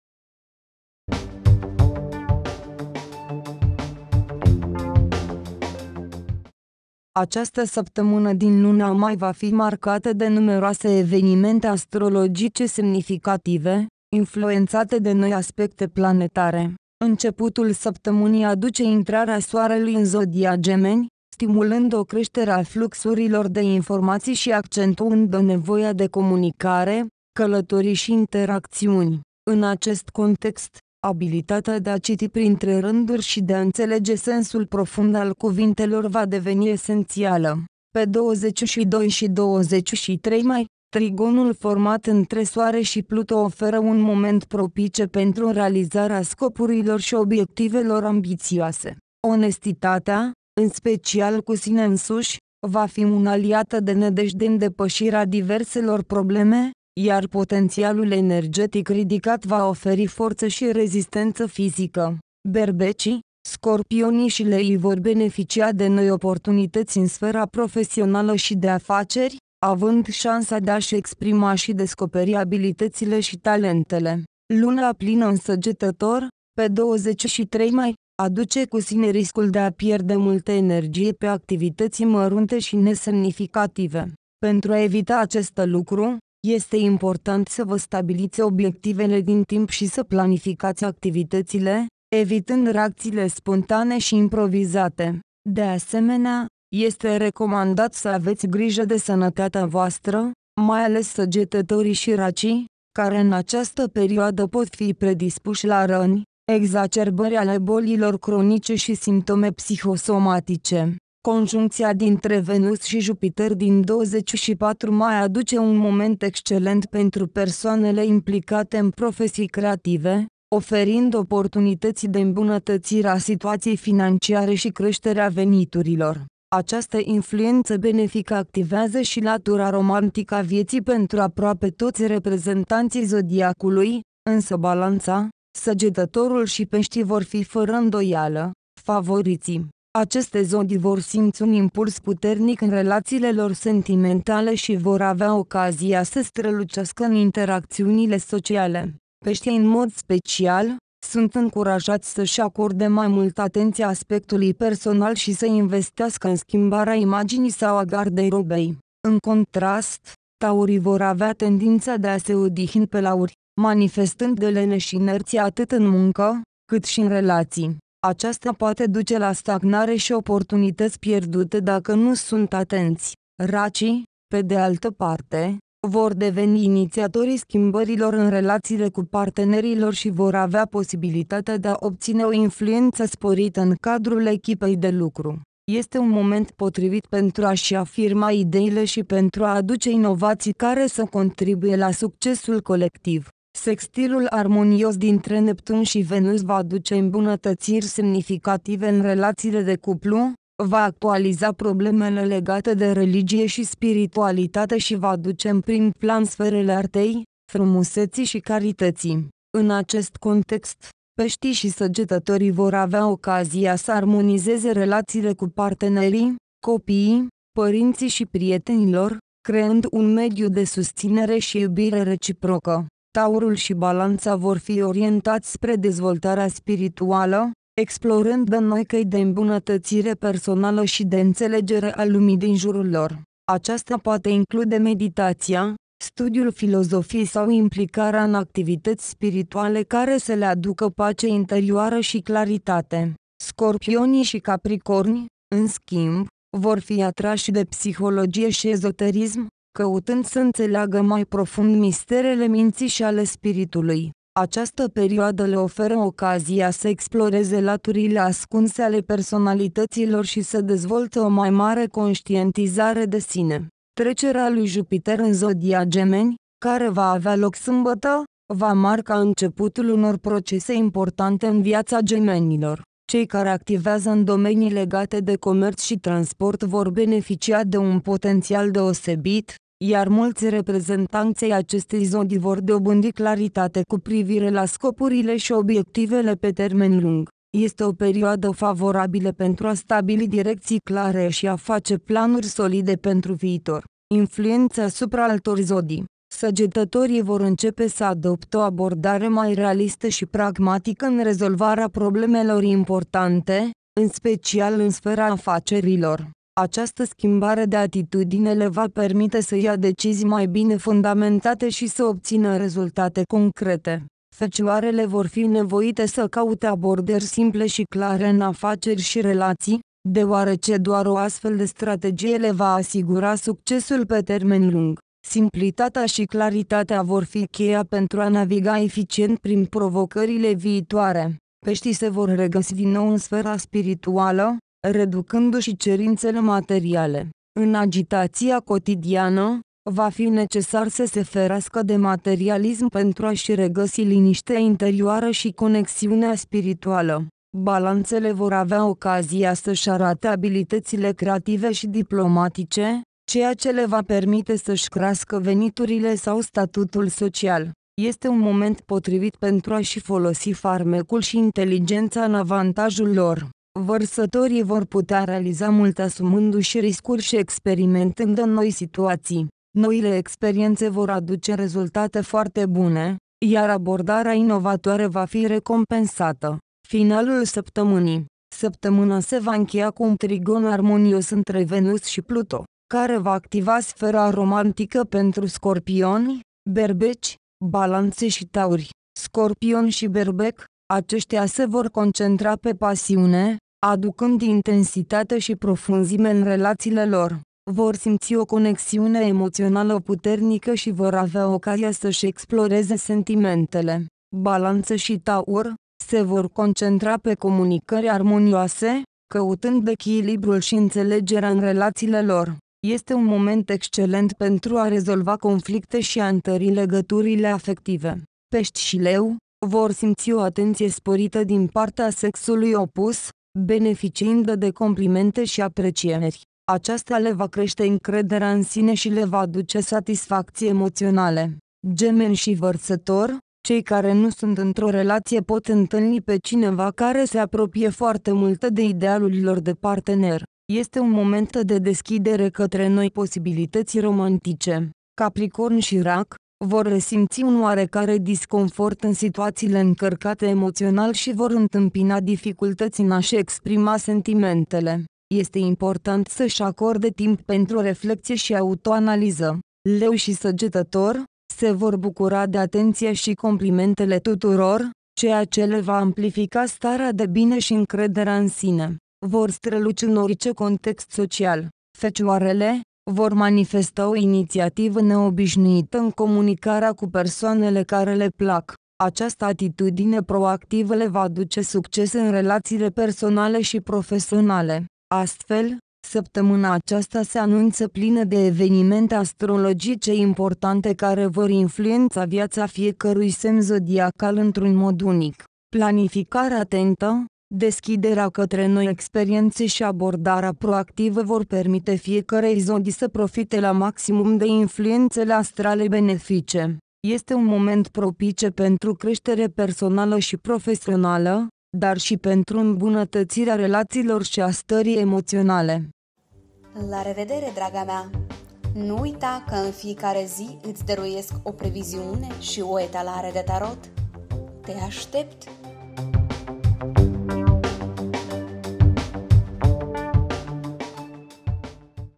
Horoscop săptămânal 20 la 26 Mai: cine un bonus în numerar și cine dragoste la prima vedere?
Te invit să ai încredere în stele și să petreci săptămâna care urmează într-o bună dispoziție și armonie.